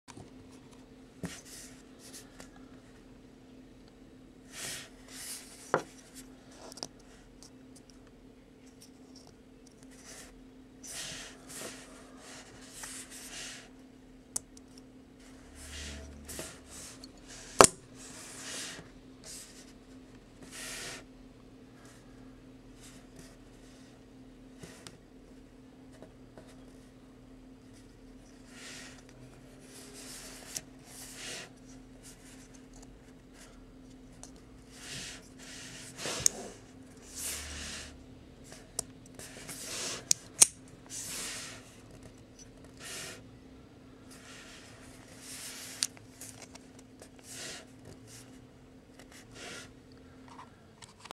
Boker Auto Mini-Magnum 117, Black Clip Point.
This is an auto conversion Boker Mini-Magnum 117.  A very nice blade.  Check it out at BladeOps.com